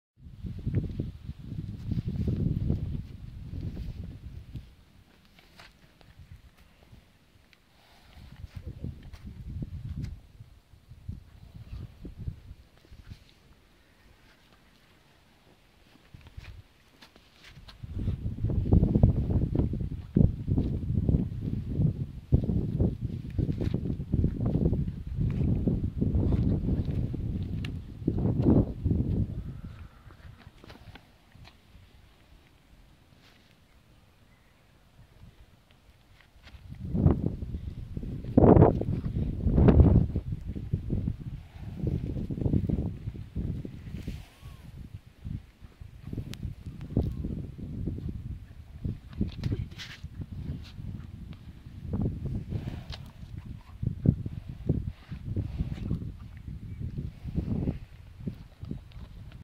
Prince Tickles
Retired Adoption horse Prince enjoying a tickle with his carers at Redwings Horse Sanctuary's HQ in Norfolk. To find out more about adopting a horse at Redwings please